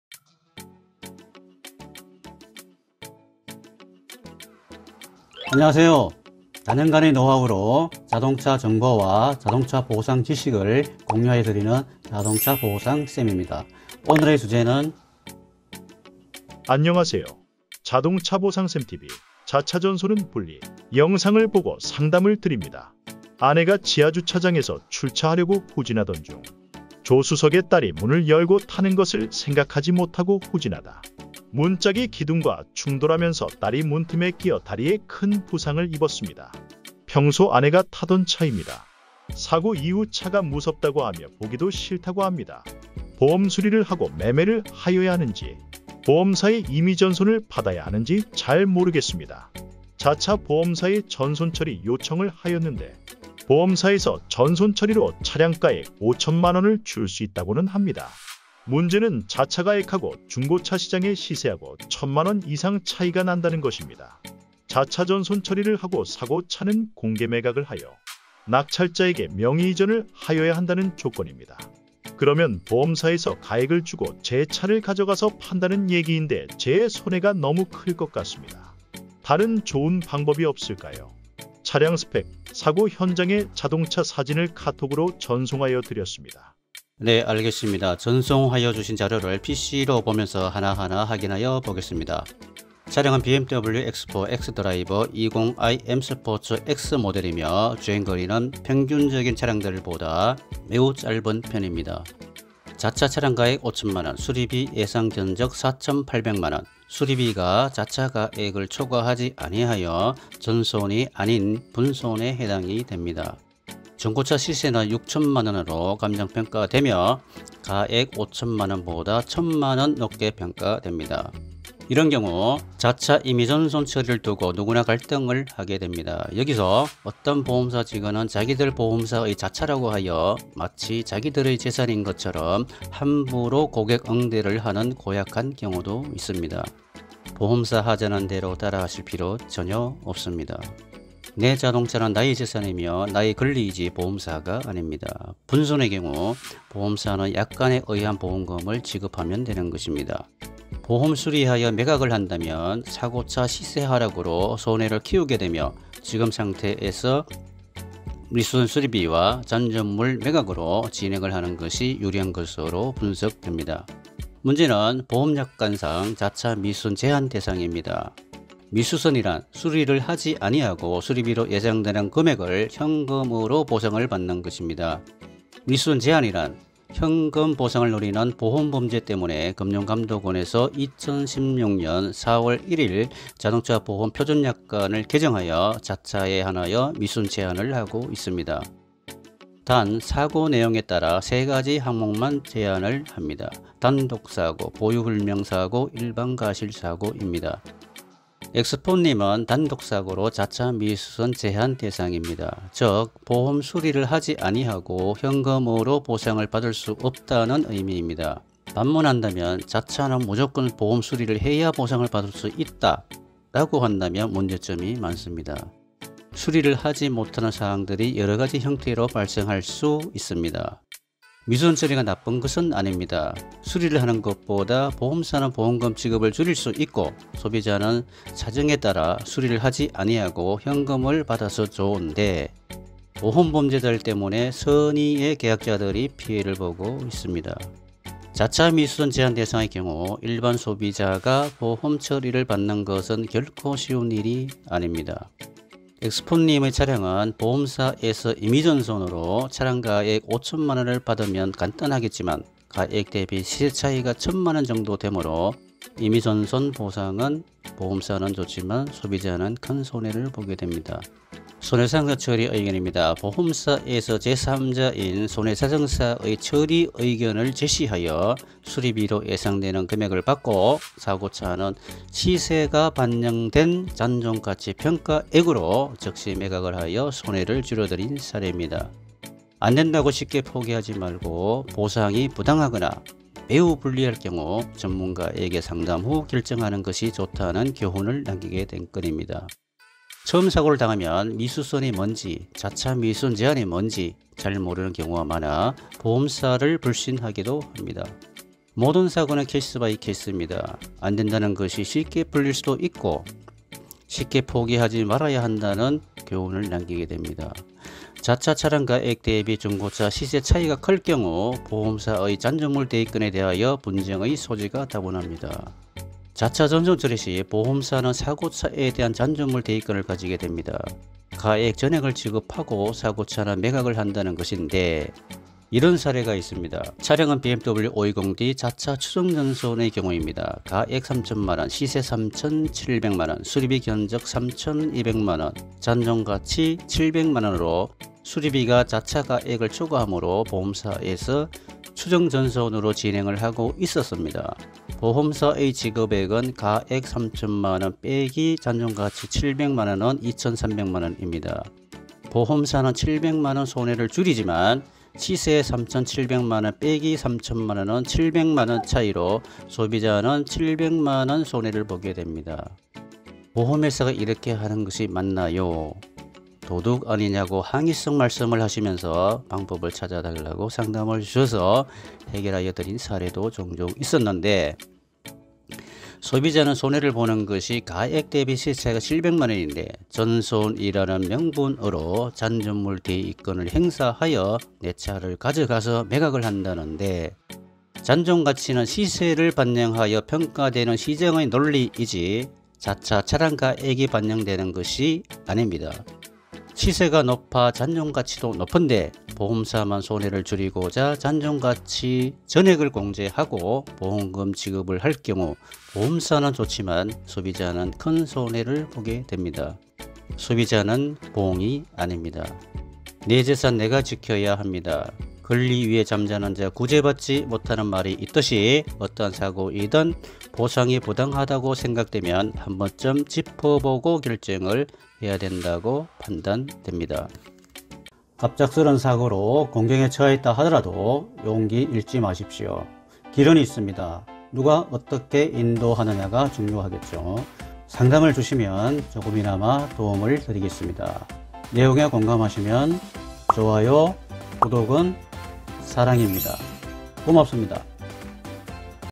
BMW X4 자차임의전손은 불리, 자차미수선제한 대상 해결방법
BMW X4 자차 미수선처리 사례입니다.

미수선제한이란~?
현금보상을 노리는
보험범죄 때문에
금융감독원에서 2016년04월01일
자동차보험 표준약관을 개정하여
자차에 한하여 미수선제한을 하고 있습니다

단, 사고내용에 따라
3가지 항목만 제한을 합니다
*단독사고
*보유불명사고
*일방과실사고
입니다.

자차미수선 제한대상의 경우
일반소비자가 보험처리를
받는 것은 결코 쉬운 일이 아닙니다.

안된다고
쉽게 포기하지 말고
보상이 부당하거나
매우 불리할 경우 전문가에게
상담 후 결정하는 것이 좋다는
교훈을 남기게 된 건입니다.

전직 손해보험 보상직원

수리, 전손, 미수선처리 비교분석 유리한 보상
손해사정,보험합의, 격락손해(시세하락손해)
[로펌의 독립손해사정사 김덕현]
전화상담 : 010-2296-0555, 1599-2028